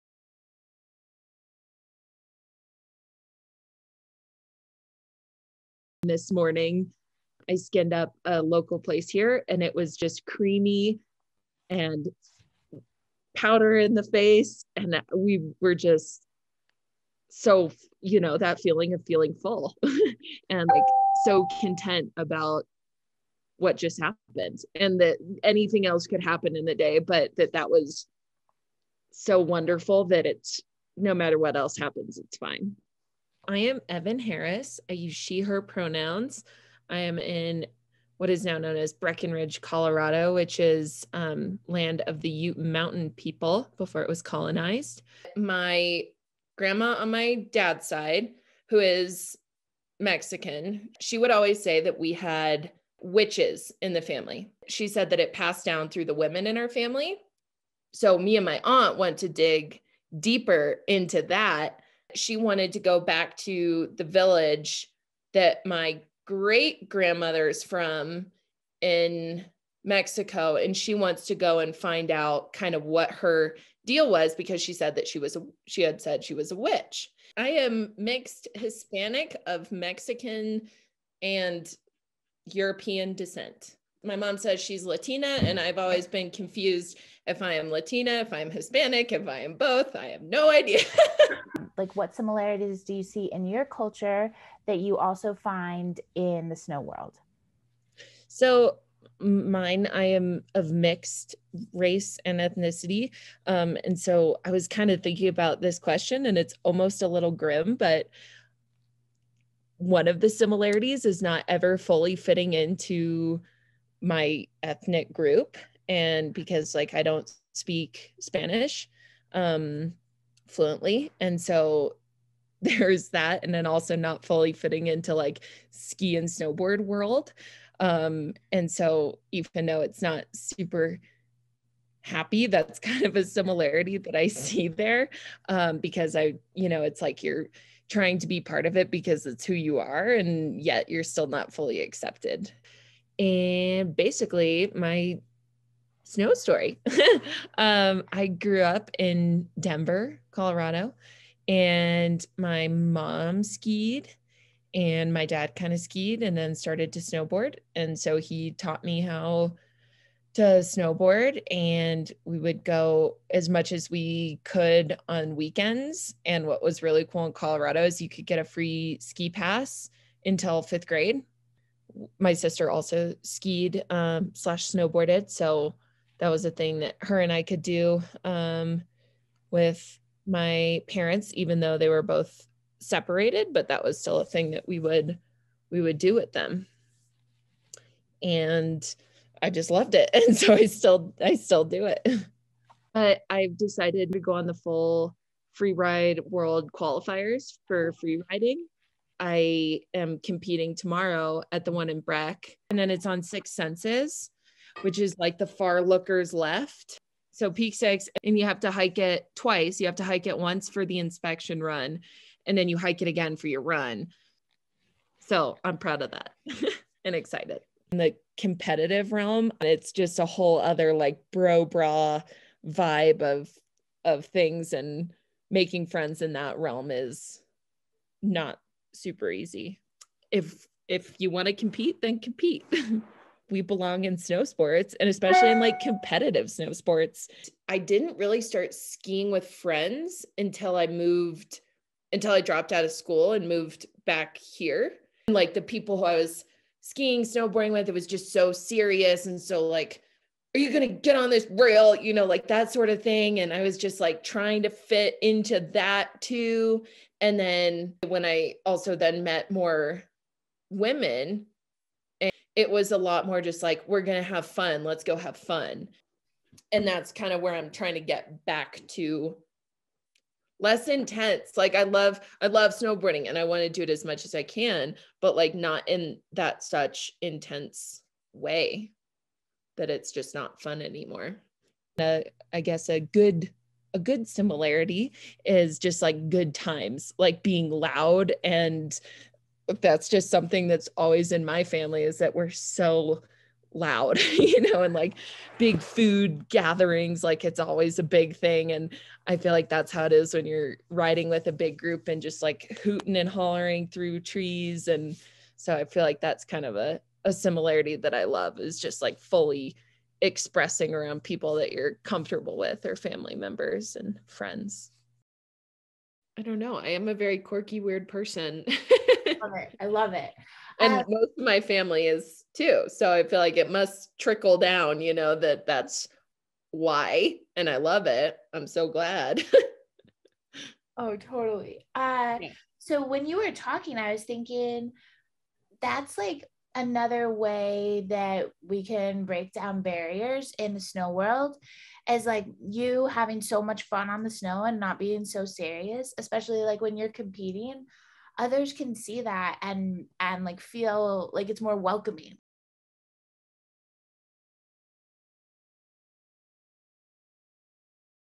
Our Lens: Mountain Stories From People of Color who Shred | Episode 5
We’ve teamed up with Liz Toft of Elevated Locals to create Our Lens, a series based on the experiences of BIPOC skiers and snowboarders. In Episode 5, we're featuring Evin Harris, a creative and competitive snowboarder based in Colorado.

"Being mixed has always felt like a struggle to 'find my people.' Snowboarding and playing in the snow have always felt right to Evin. Meeting people, especially women who have similar experiences is especially uplifting. There is space in snowsports for everyone."

"Our Lens" is a series about their rarely told and rarely represented mountain stories. Liz has compiled, shot, and edited footage from our Mountaintop members to tell their mountain stories and help create more visibility for under-represented skiers and snowboarders within the snowsport community. Liz describes the series as “A collection of snow vision stories celebrating the diverse, radiant perspectives of the Mountaintop BIPOC community. This project weaves together individual experiences while exploring larger themes distinct to members of the BIPOC community.”